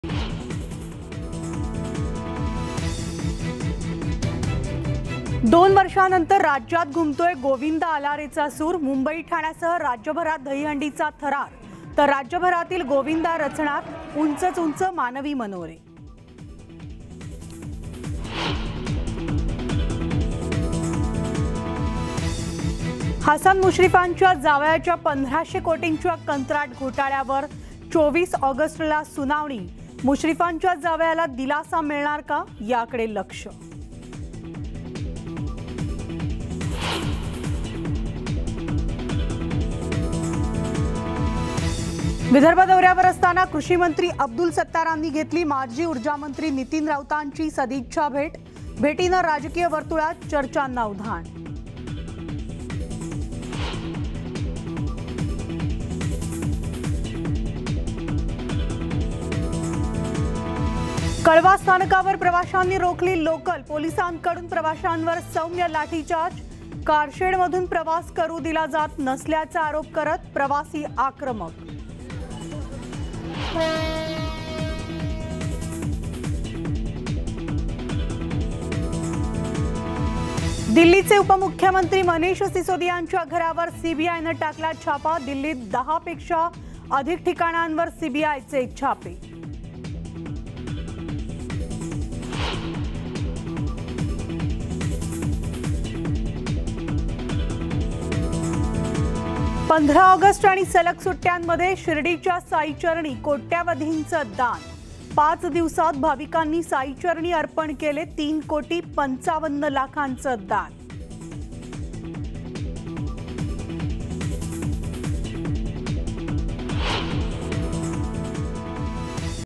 11 AM Top Headlines | Marathi News | News18 Lokmat | Aug 19, 2022
After two years of the Covid-induced restrictions, the Dahi Handi festival will be celebrated on a grand scale in Maharashtra, keeping all coronavirus-related guidelines in mind.

कोरोनानंतर 2 वर्षांनी दहीहंडीचा जल्लोष आणि उत्साह राज्यभरात आहे. मुंबई-ठाण्यात यंदा हा उत्साह अधिक जास्त आहे.

n18oc_mumbai